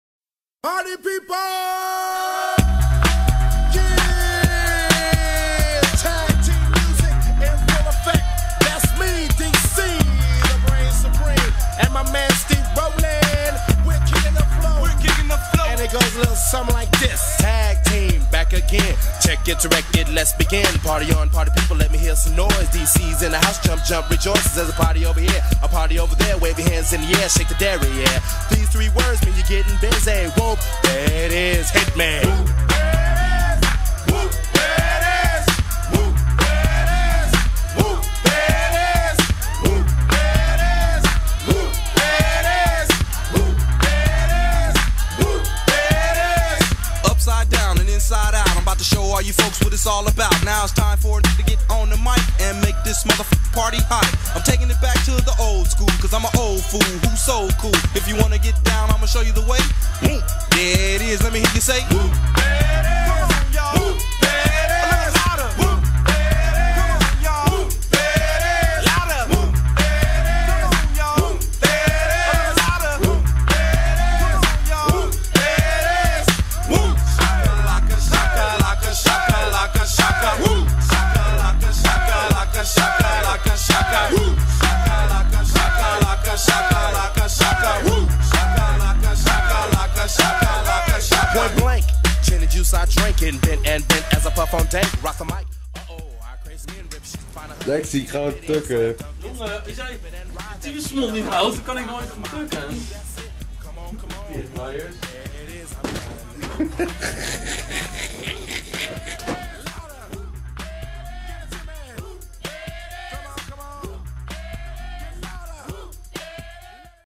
Pokercity.nl @ WSOP 2008
Lex bereid zich voor op dag 2.